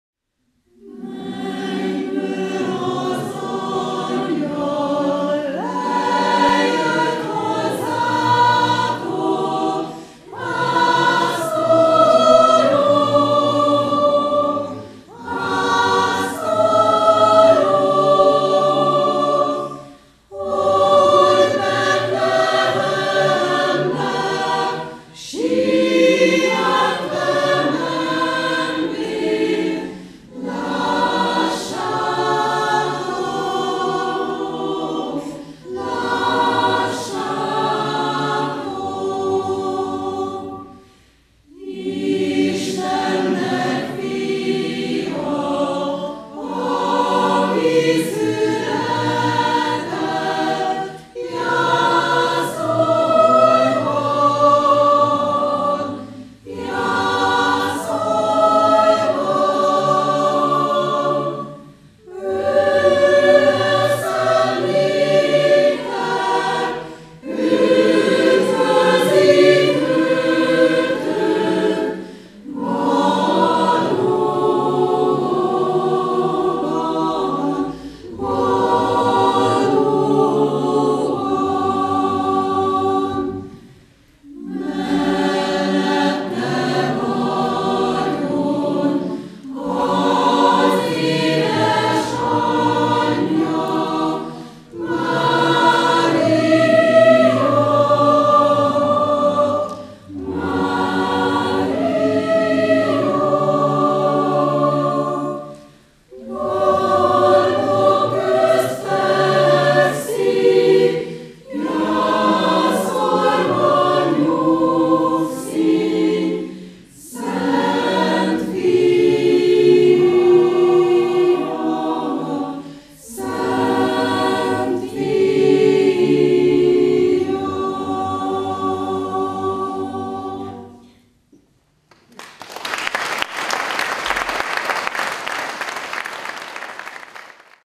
Ajánló! - Adventi zenés áhítat a kistarcsai Vivace kórussal
Körzeműködnek:
Scola és a zenekar tagjai

Vezényel:
Kissné Bozó Sarolta

Kistarcsa, Rózsafüzér Királynője templom
2015. december 13